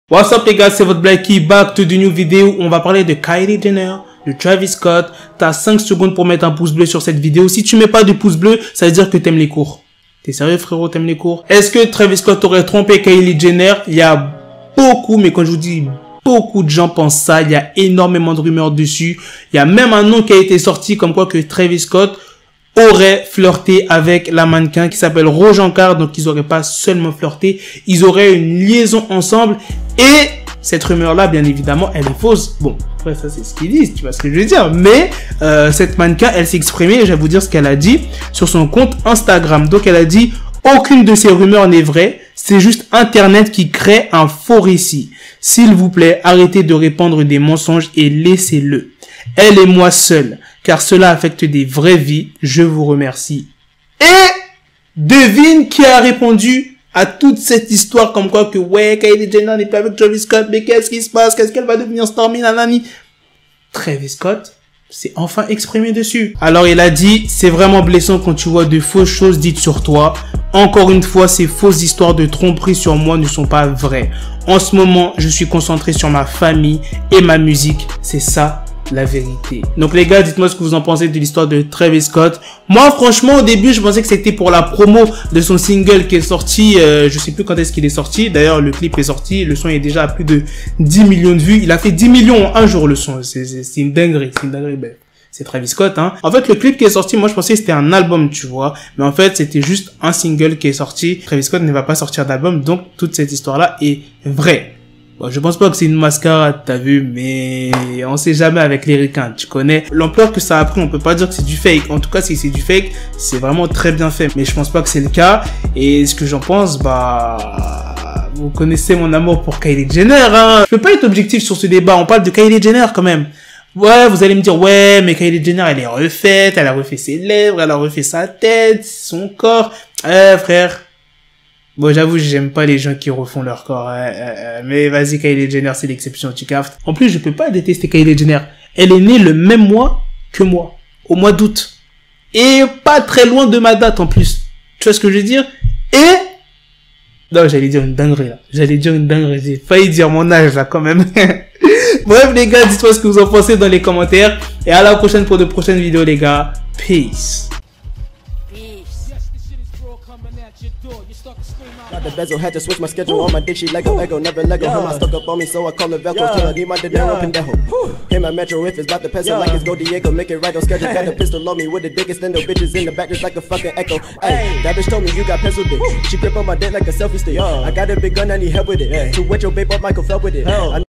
TRAVIS SCOTT A TROMPÉ KYLIE JENNER AVEC CETTE PERSONNE ? IL S'EXPLIQUE
Travis Scott
Kylie Jenner
tromperie
couple